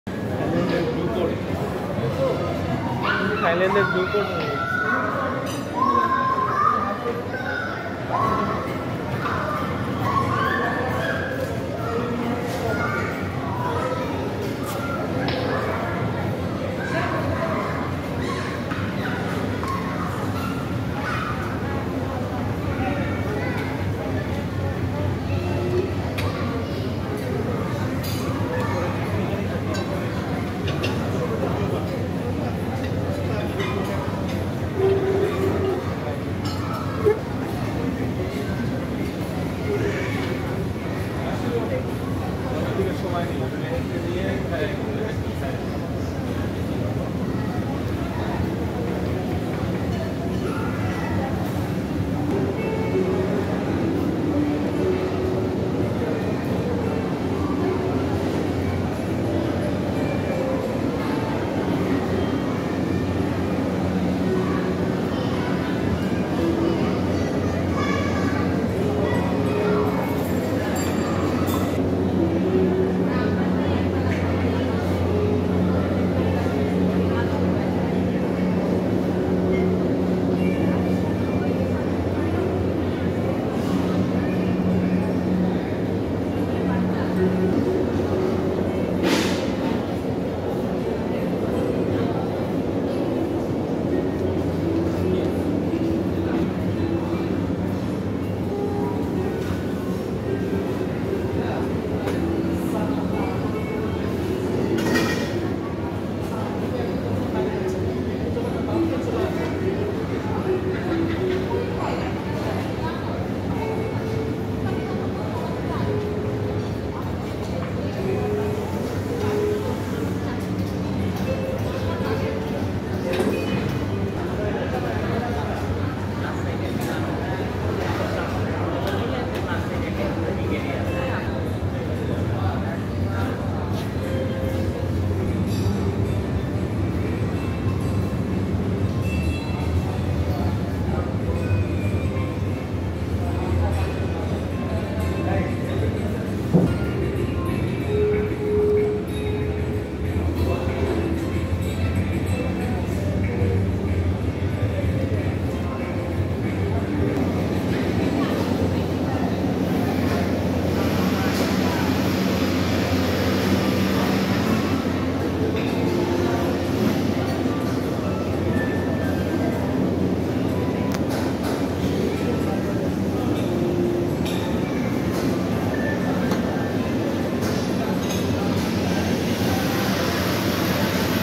Chef's Table, Centre Point
The country's biggest Chef's Table now at Uttara. This is located at centrepoint just after Hazrar Sahajalal International Airport.